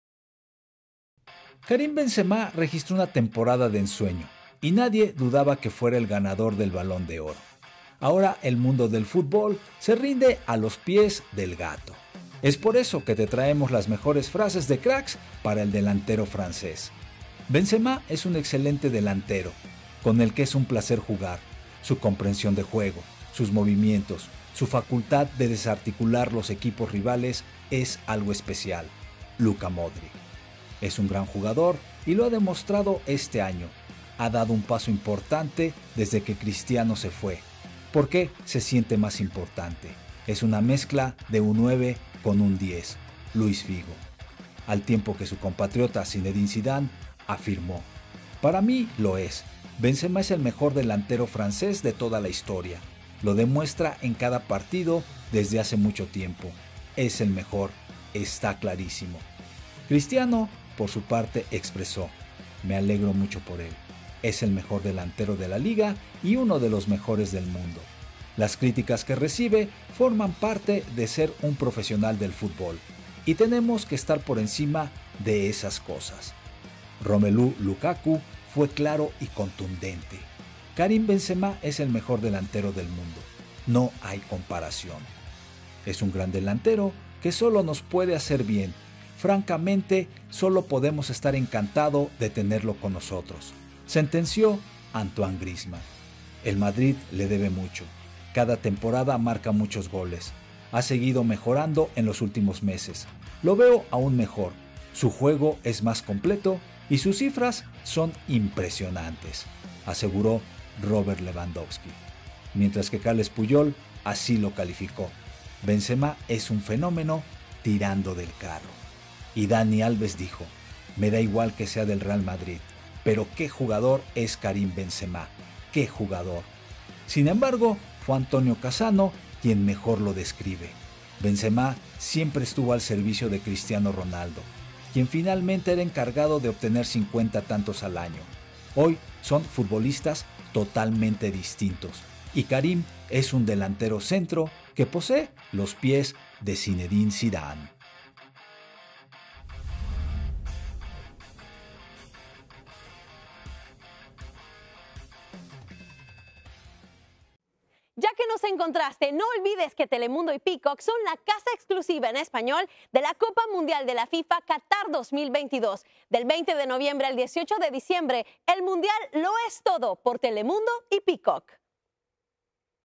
Karim Benzema: El mundo se deshace en elogios para el nuevo Balón de Oro | Telemundo Deportes
Video oficial de Telemundo Deportes. Nadie dudaba que el delantero francés Karim Benzema se adjudicara el Balón de Oro y por eso el mundo del fútbol se rinde a sus pies.